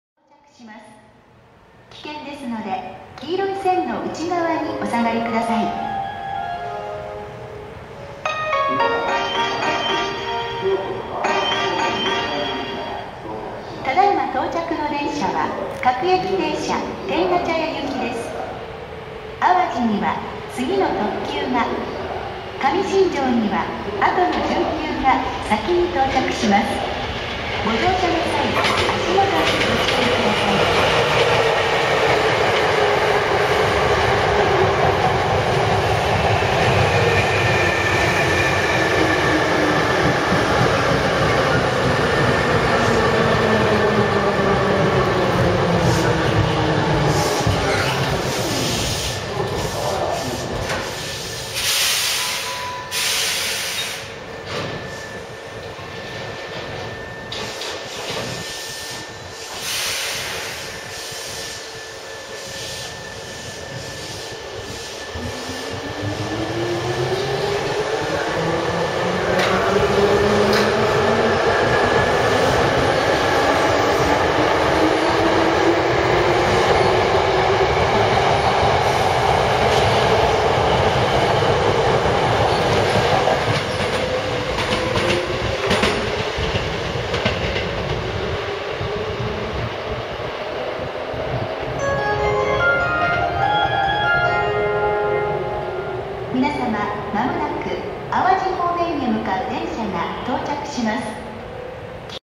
【駅自動放送つき】阪急京都線茨木市駅 5300系準急河原町行き到着と大阪市営地下鉄堺筋線直通普通天下茶屋行き発車
阪急京都本線茨木市駅2号線に到着する準急河原町行と、3号線を発車する大阪市営地下鉄堺筋線直通の普通天下茶屋行。どちらも5300系8両編成。駅自動放送も収録。

＝2013/11/17撮影（Panasonic HX-DC2）＝